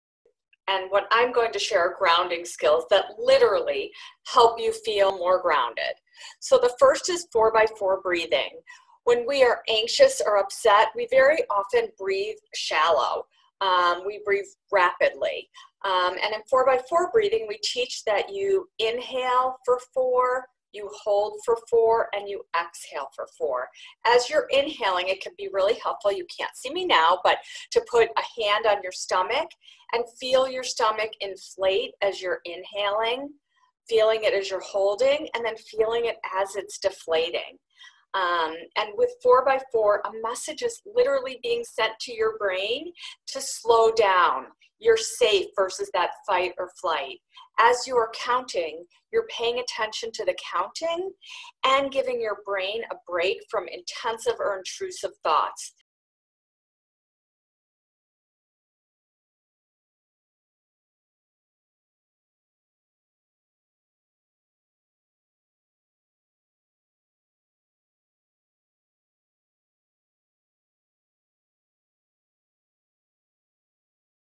4x4 Breathing [Anxiety Grounding Tool] | CATCH Clip
Just breathe.  It really can make a difference for children and parents when anxiety takes hold.  In this Catch Clip, Roz Lessem,  LCPC, Compass Health Center, Northbrook, IL, explains the grounding tool: 4x4 Breathing. Watch, learn, and add this go-to-strategy to your toolkit for coping with anxiety.

This CATCH Clip first appeared in the webinar:  Managing the Flood of Emotions Brought on by a Pandemic: Supporting Our Children's Ongoing Needs, a collaboration between CATCH and Compass Health Center